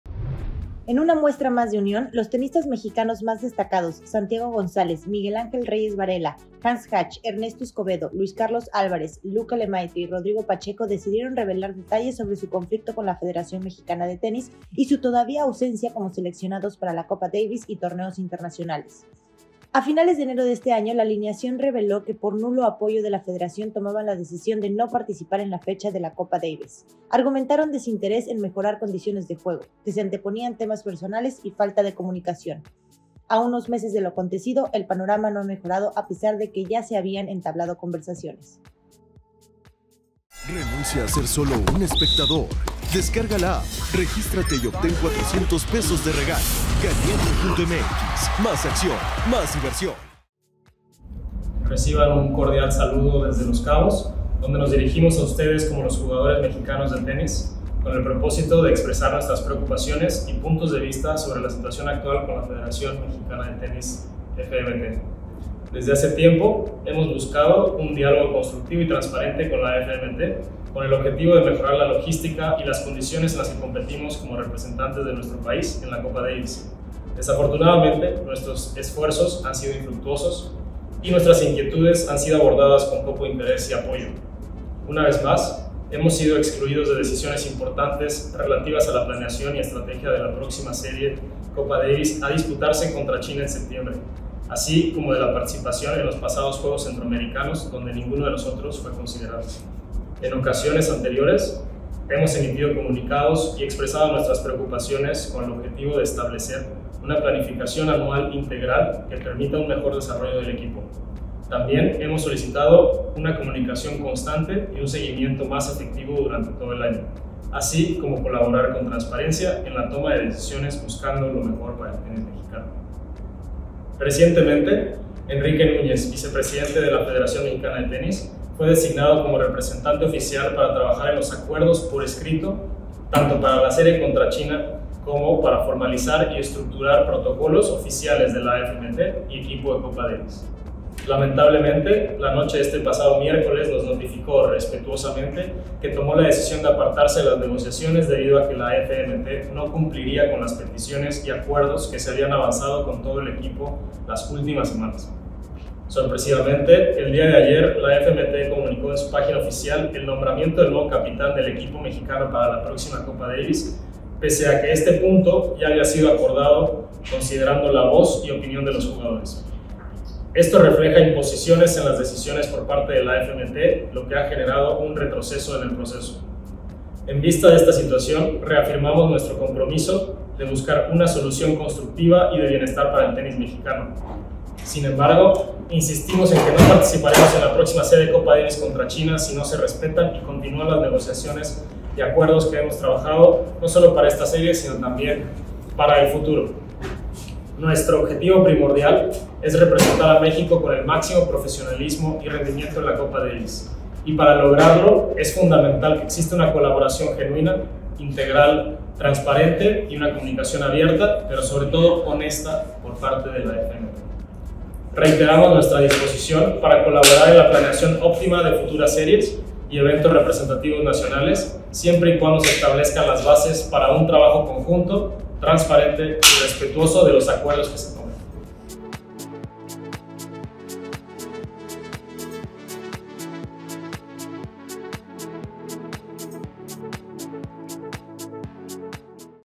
Tenistas mexicanos, en conflicto con la Federación Mexicana de Tenis
Tenistas mexicanos, en conflicto con la Federación Mexicana de Tenis - En una muestra más de unión, los tenistas mexicanos más destacados Santiago González, Miguel Àngel Reyes-Varela, Hans Hach, Ernesto Escobedo, Luis Carlos Álvarez, Luca Lemaitre y Rodrigo Pacheco decidieron revelar detalles sobre su conflicto con la Federación Mexicana de Tenis y su todavía ausencia como seleccionados para la Copa Davis y torneos internacionales.

A finales de enero de este año, la alineación reveló que por nulo apoyo de la FMT tomaban la decisión de no participar en la fecha de la Copa Davis. Argumentaron desinterés en mejorar condiciones de juego, que se anteponían temas personales y falta de comunicación. A unos meses de lo acontecido, el panorama no ha mejorado a pesar de que ya se habían entablado conversaciones.